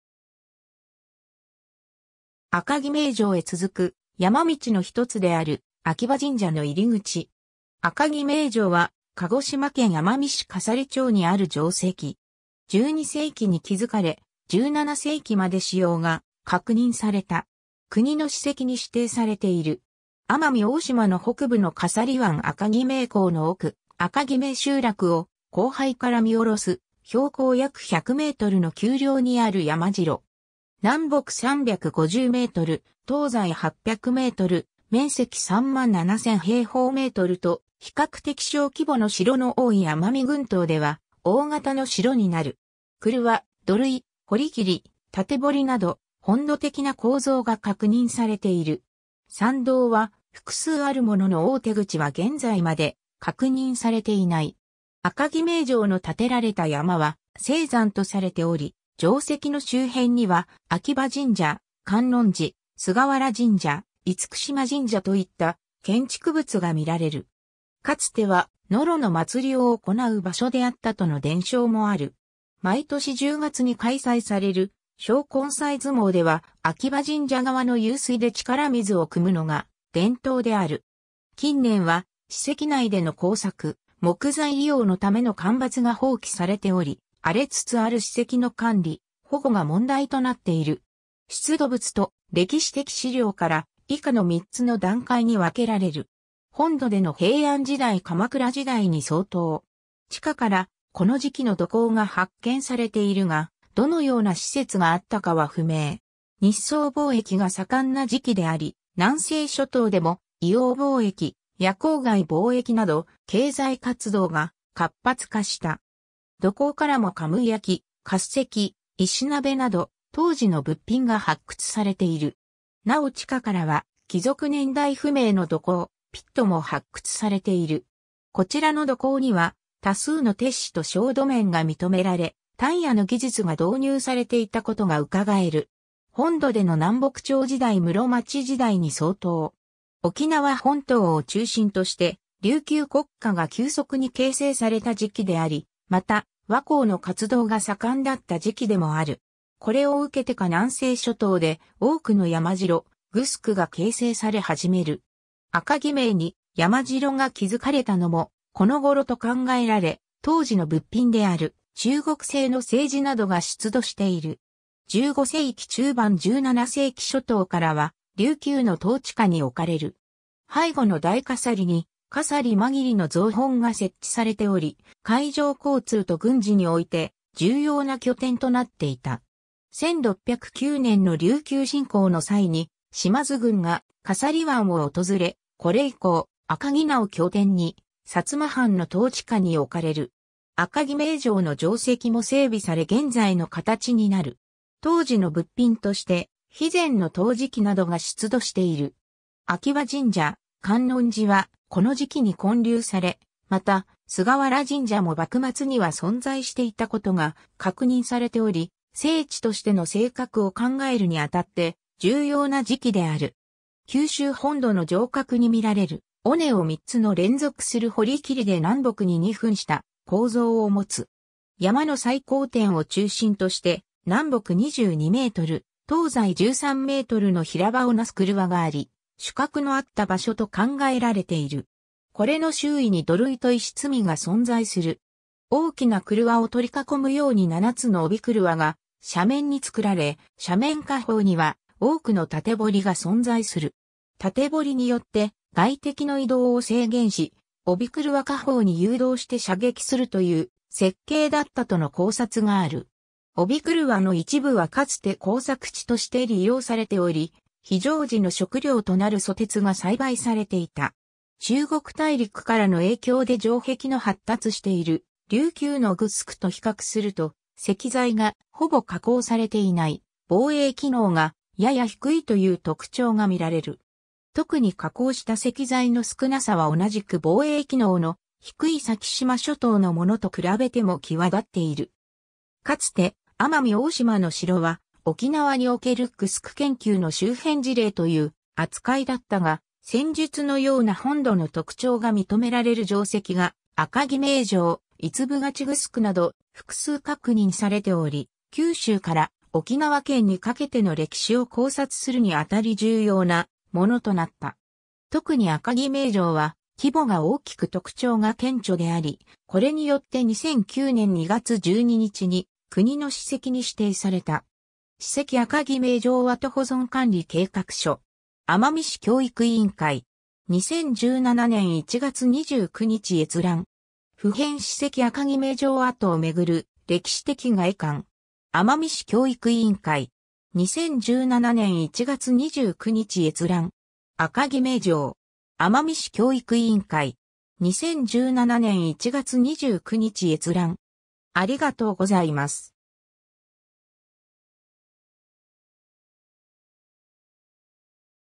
赤木名城
赤木名城へ続く山道の一つである秋葉神社の入り口。
 赤木名城（あかきなじょう）は、鹿児島県奄美市笠利町（奄美群島奄美大島）にある城跡。
12世紀に築かれ、17世紀まで使用が確認された。
 奄美大島の北部の笠利湾赤木名港の奥、赤木名集落を後背から見下ろす標高約100メートルの丘陵にある山城。
南北350m、東西800m、面積37000m2と比較的小規模の城の多い奄美群島では大型の城になる。
曲輪、土塁、堀切、竪堀など本土的な構造が確認されている。
山道は複数あるものの大手口は現在まで確認されていない。
 赤木名城の建てられた山は聖山とされており、城跡の周辺には秋葉神社・観音寺(移転済み)・菅原神社・厳島神社といった建築物が見られる。
かつてはノロの祭を行う場所であったとの伝承もある。
毎年10月に開催される招魂祭相撲では秋葉神社側の湧水で力水を汲むのが伝統である。
 近年は史跡内での耕作・木材利用のための間伐が放棄されており、荒れつつある史跡の管理・保護が問題となっている。
 出土物と歴史的資料から以下の三つの段階に分けられる。
 本土での平安時代〜鎌倉時代に相当。
地下からこの時期の土坑が発見されているがどのような施設があったかは不明。
 日宋貿易が盛んな時期であり南西諸島でも硫黄貿易・夜光貝貿易など経済活動が活発化した。
土坑からもカムィヤキ・滑石・石鍋など当時の物品が発掘されている。
 なお地下からは帰属年代不明の土坑・ピットも発掘されている。
こちらの土坑には多数の鉄滓と焦土面が認められ、鍛冶の技術が導入されていたことが伺える。
 本土での南北朝時代〜室町時代に相当。
 沖縄本島を中心として琉球国家が急速に形成された時期であり、また倭寇の活動が盛んだった時期でもある。
これを受けてか南西諸島で多くの山城・グスクが形成され始める。
赤木名に山城が築かれたのもこの頃と考えられ、当時の物品である中国製の青磁などが出土している。
 15世紀中盤〜17世紀初頭からは琉球の統治下に置かれる。
背後の大笠利に笠利間切の蔵本が設置されており、海上交通と軍事において重要な拠点となっていた。
 1609年の琉球侵攻の際に島津軍が笠利湾を訪れ、これ以降赤木名を拠点に薩摩藩の統治下に置かれる。
赤木名城の城跡も整備され現在の形になる。
当時の物品として肥前の陶磁器などが出土している。
 秋葉神社・観音寺はこの時期に建立され、また菅原神社も幕末には存在していたことが確認されており、聖地としての性格を考えるにあたって重要な時期である。
 九州本土の城郭に見られる、尾根を三つの連続する堀切で南北に二分した構造を持つ。
 山の最高点を中心として南北22m・東西13mの平場をなす曲輪があり、主郭のあった場所と考えられている。
これの周囲に土塁と石積みが存在する。
 大きな曲輪を取り囲むように七つの帯曲輪が斜面に作られ、斜面下方には多くの竪堀が存在する。
竪堀によって外敵の移動を制限し、帯曲輪下方に誘導して射撃するという設計だったとの考察がある。
帯曲輪の一部はかつて耕作地として利用されており、非常時の食料となるソテツが栽培されていた。
 中国大陸からの影響で城壁の発達している琉球のグスクと比較すると、石材がほぼ加工されていない・防衛機能がやや低いという特徴が見られる。
特に加工した石材の少なさは同じく防衛機能の低い先島諸島のものと比べても際立っている。
 かつて奄美大島の城は沖縄におけるグスク研究の周辺事例という扱いだったが、先述のような本土の特徴が認められる城跡が赤木名城・伊津部勝グスクなど複数確認されており、九州から沖縄県にかけての歴史を考察するにあたり重要なものとなった。
特に赤木名城は規模が大きく特徴が顕著であり、これによって2009年（平成21年）2月12日に国の史跡に指定された。
 “史跡赤木名城跡保存管理計画書 (PDF)”. 奄美市教育委員会 (2015年3月31日). 2017年1月29日閲覧。
 “付編 史跡赤木名城跡をめぐる歴史的概観 (PDF)”. 奄美市教育委員会 (2015年3月31日). 2017年1月29日閲覧。
 “赤木名城 (PDF)”. 奄美市教育委員会 (2008年3月31日). 2017年1月29日閲覧。